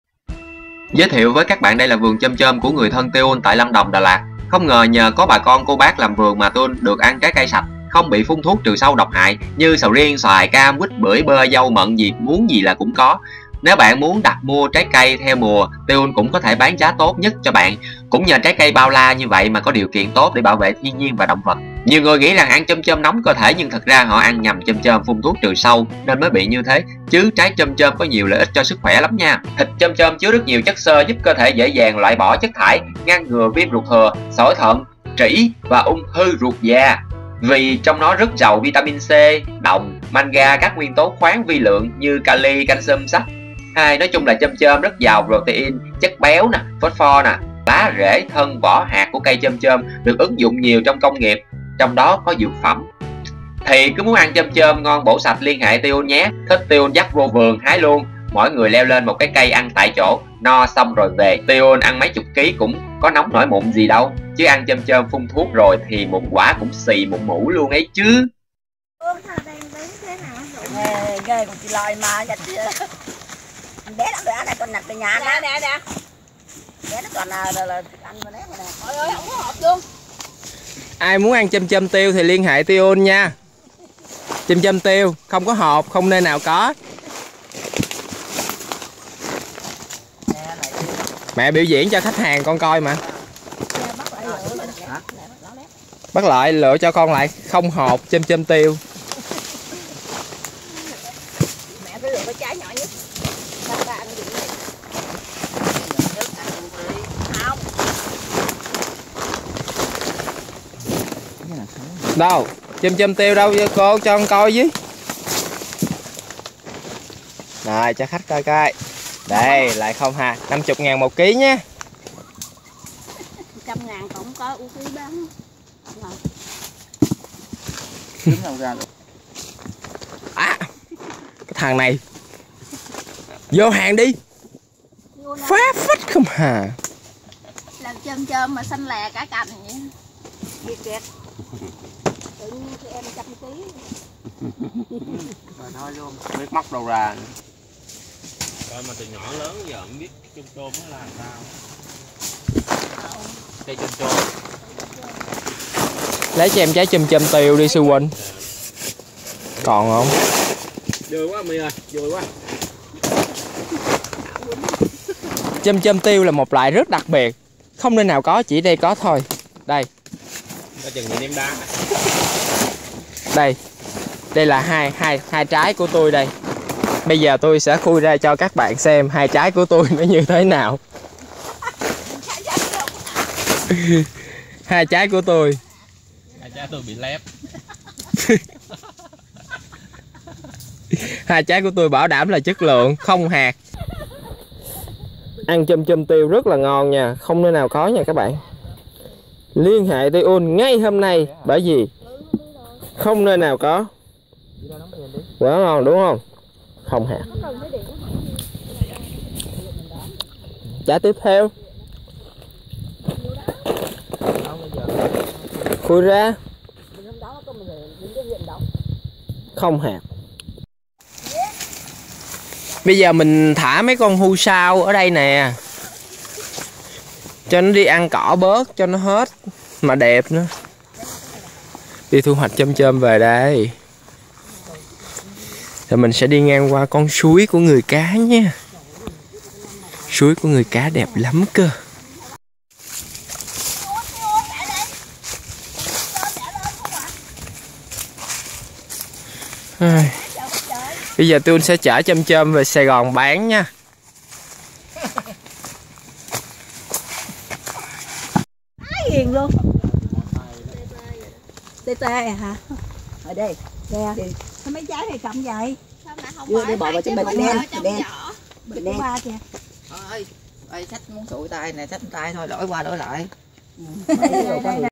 Ai Ngờ Chôm Chôm Rẻ Tiền Mà Lại Quý Hơn Tiên Dược - Tiun
Native to the Malay Archipelago, the name of this fruit is derived from the Malay word meaning "hairy," and you can see why. But once the hairy exterior of the rambutan is peeled away, the tender, fleshy, delicious fruit is revealed. Its taste is described as sweet and sour, much like a grape. Though it has its origin in Southeast Asia, rambutan has been imported around the world, and now is commonly cultivated as close to home as Mexico and Hawaii.

Đâu cần làm đại gia, mỗi nhà có một vườn trái cây ăn hoài 3 đời cũng không chết đói. Rất nhiều người thích ăn chôm chôm nhưng ít người khám phá ra công dụng thần kỳ của quả chôm chôm có rất nhiều lợi ích với sức khỏe. Thịt chôm chôm chứa rất nhiều chất xơ giúp cơ thể dễ dàng loại bỏ chất thải, tiêu diệt ký sinh trùng, loại bỏ độc tố trong thận, tưa miệng, bổ máu, xoa dịu thần kinh và làm giảm đau đầu rất hiệu quả, chữa lỵ, làm đẹp, giảm cân. Hiện tại chôm chôm bán ở chợ đều có phun thuốc trừ sâu nên ăn vào bạn sẽ thấy đắng. Còn chôm chôm vườn nhà Tiun trồng là xuất khẩu bán cho nước ngoài, nên phải đảm bảo quy trình chuyên nghiệp hơn. Tội nghiệp người Việt Nam, thiên nhiên của ta mà không ăn được trái cây ngon của ta, phải ăn trái cây Trung Quốc độc hại. Bởi vậy mới thấy đời sống mấy người sống trong thành phố thua mấy cô chú làm vườn dưới quê, không có rau củ quả sạch để ăn, có tiền mà ăn toàn đồ độc hại, rồi vài năm phát bịnh ung thư nhập viện, đúng khổ luôn. Chỉ vì cái tội không biết bảo vệ thiên nhiên, có tiền bán đất xây nhà mà không chịu trồng cây.